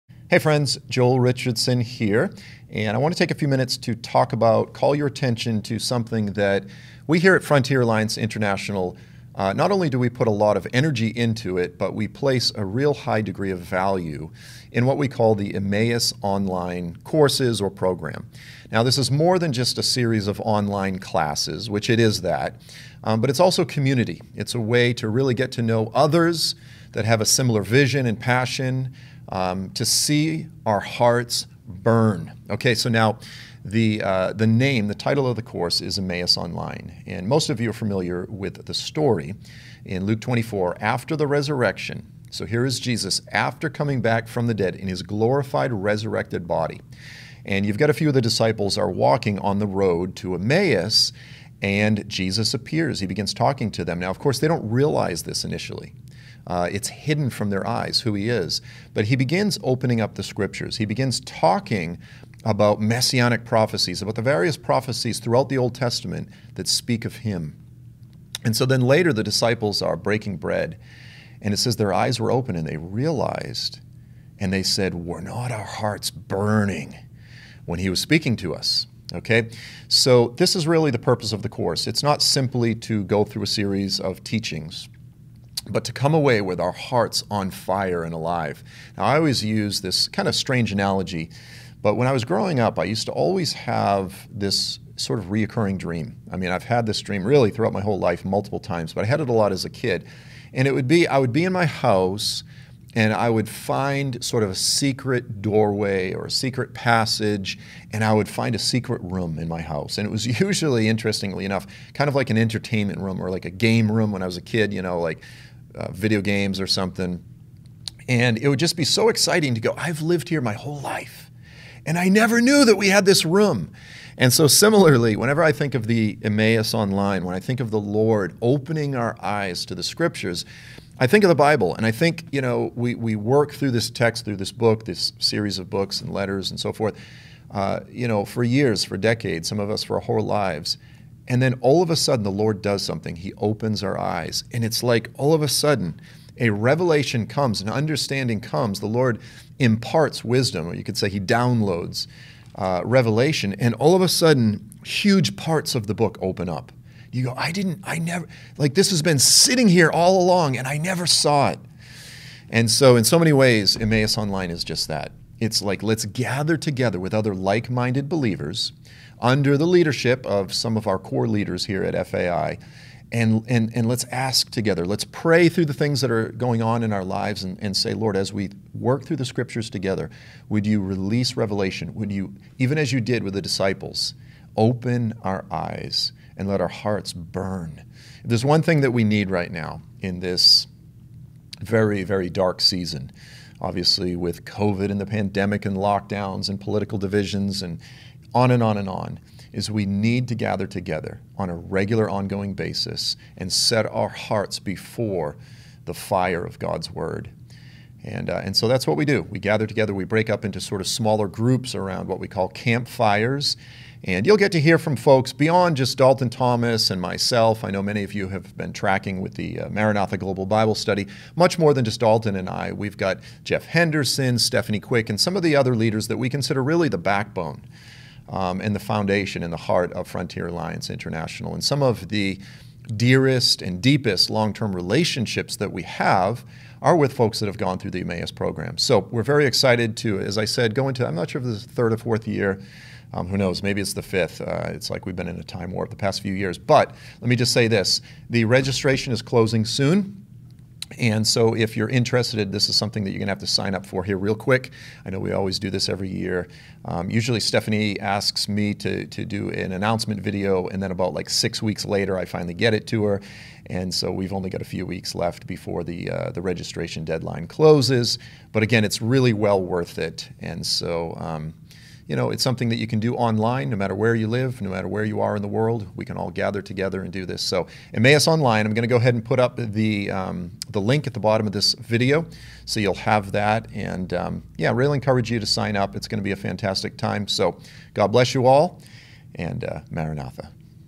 EMMAUS ONLINE // 2022 ENROLLMENT
Wherever you are and whatever you do, if you love Jesus, if you care about the unreached, if you look forward to Jesus’ return, this eight-month course is for you. Emmaus Online will equip you as a leader, encourage you as a student of the Word, and empower you as a disciple of Jesus.

Enrollment ends February 21, 2022.